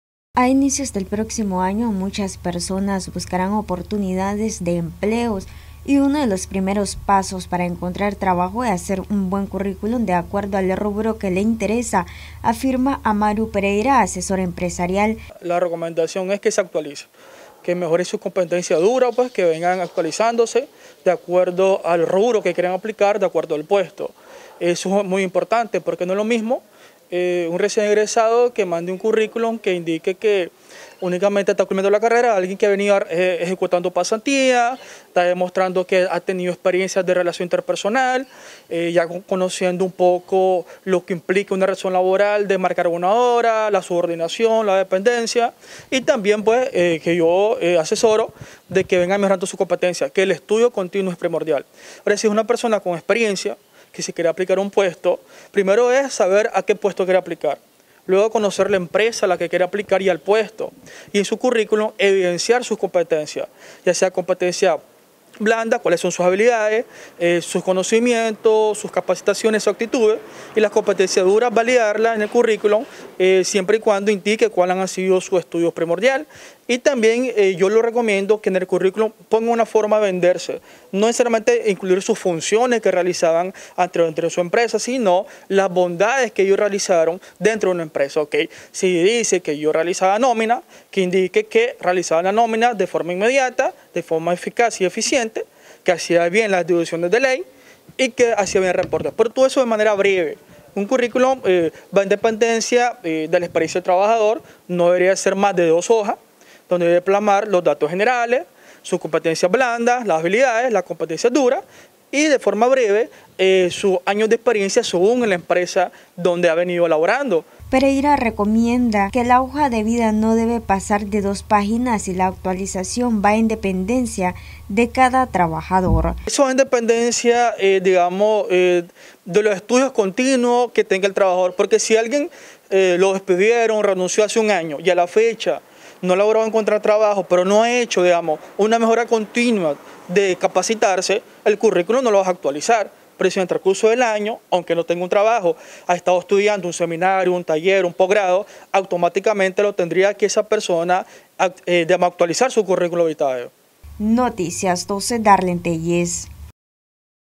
Solicitudes de empleo deben ir acompañadas de un buen currículum
Noticias12Nicaragua Asesor empresarial da a conocer algunas recomendaciones que deben implementar en su currículo para aquellas personas que buscarán empleo.